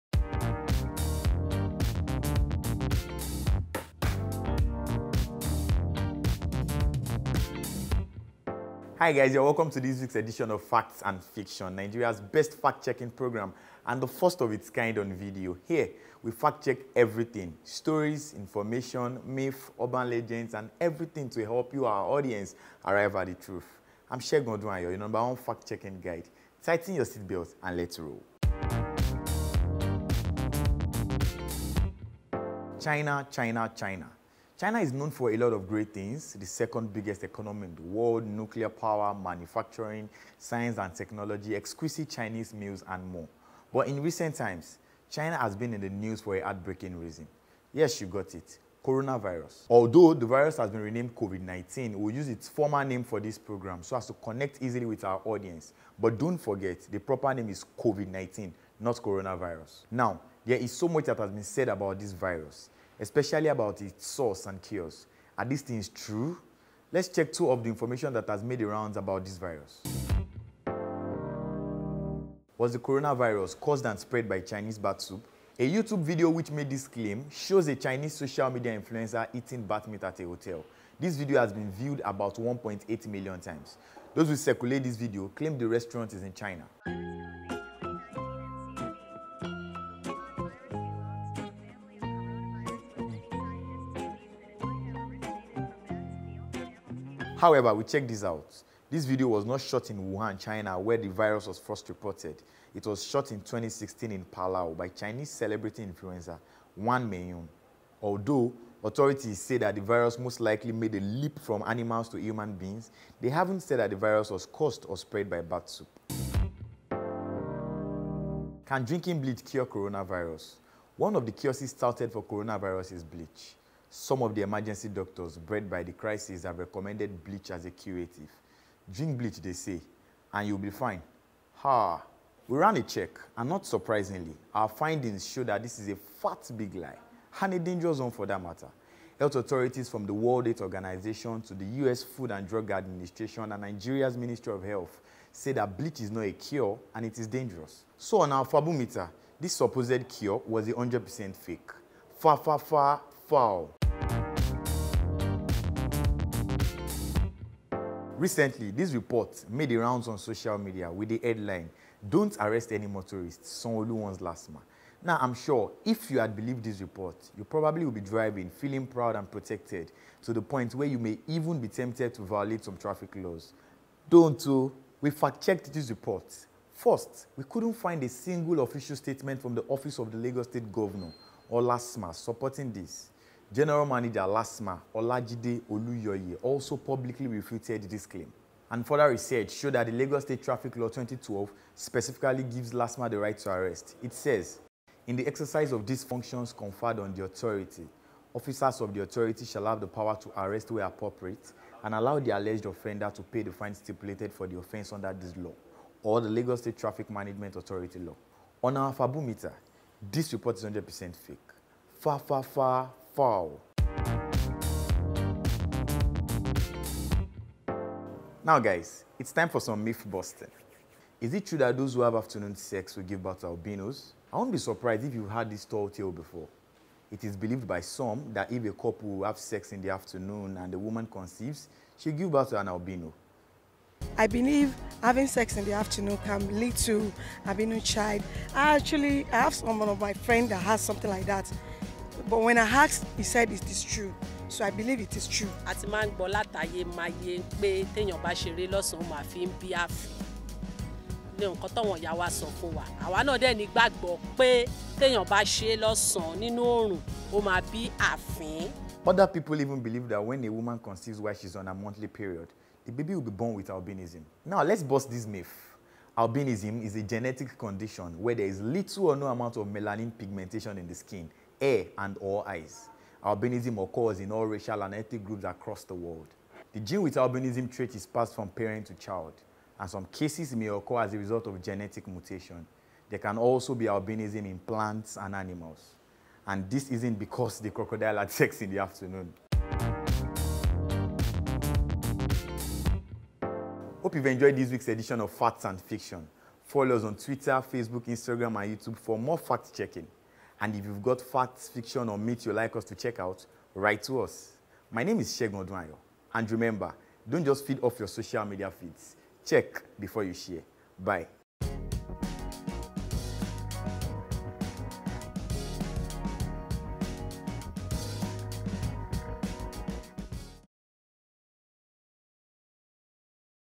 Facts and Fiction: Did Sanwo-Olu Order LASTMA To Stop Arresting Motorists? | Punch
This second episode of our fact-checking series is delving into the world of Coronavirus, now formally named Covid 19 and rumours about its source and how to prevent it.

Also did the governor of Lagos State ask operatives of the state traffic management agency, LASTMA, not to arrest motorists?

Better watch this video before you drive on Lagos roads.

Now to some myths, is it true that those who have sex in the afternoon end up having albinos? The answer to this myth lies in this video.